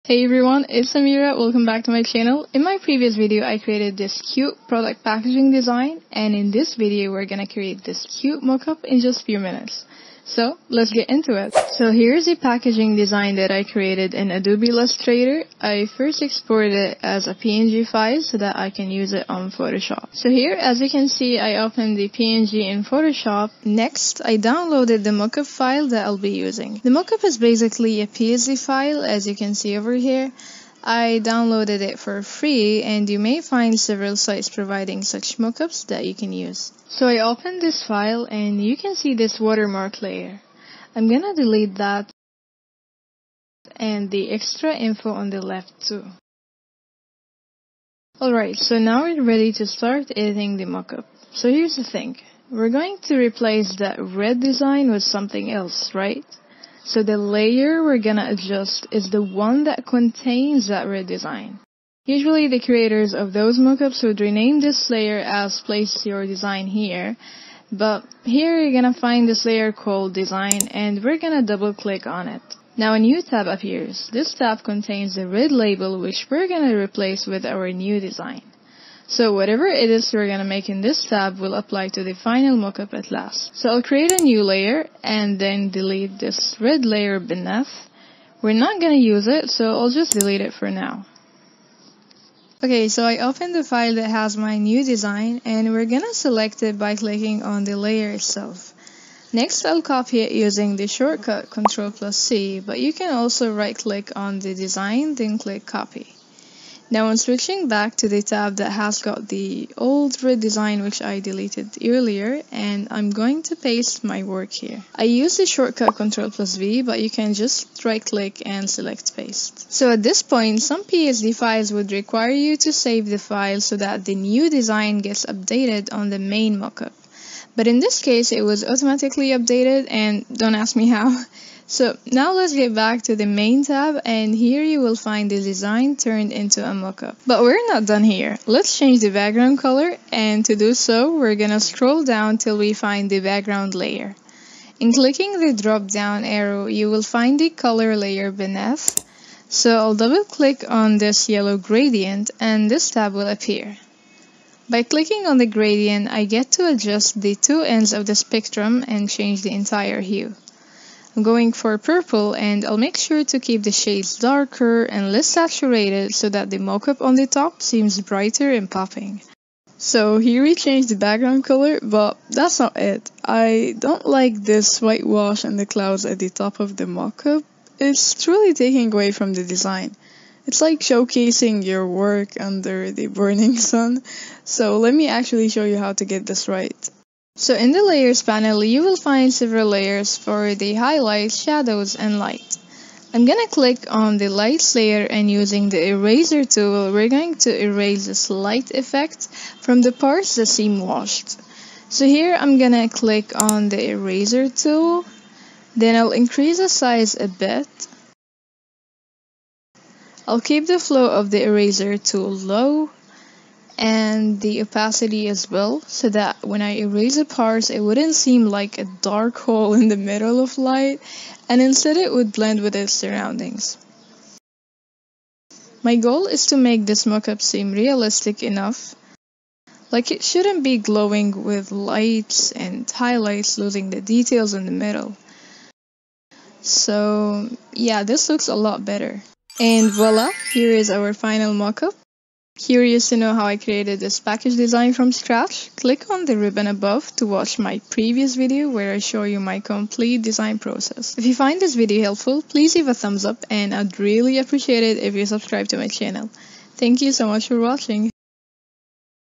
How to Create a Mockup in Photoshop- Tutorial | FOR BEGINNERS!
Hey guys! In my previous video I show you how I designed this packaging design from scratch, and in this video, I'll show you how to create mockups in photoshop. When I first started out my product packaging journey, I felt a bit overwhelmed when I had to create mockups.. BUT.. I found that it's actually exciting once you know the basics :)
If you find this video helpful, thumbs up this video!